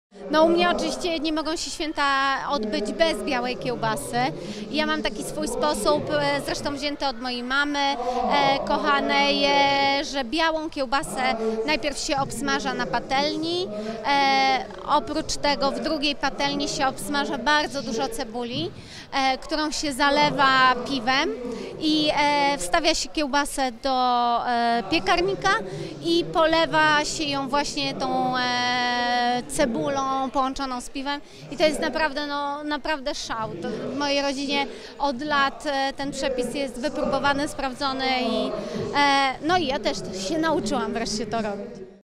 Agnieszka Jastrzębska zdradza przepis rodzinny na Wielkanocne danie
Święta wielkanocne już za kilka dni. Tymczasem w niemalże wszystkich domach trwają wielkie przygotowywania – sprzątanie, dekorowanie i… obmyślanie świątecznego menu. Niektórzy jak co roku wykorzystają sprawdzone, rodzinne przepisy, a inni – spróbują unowocześnić tradycyjne potrawy.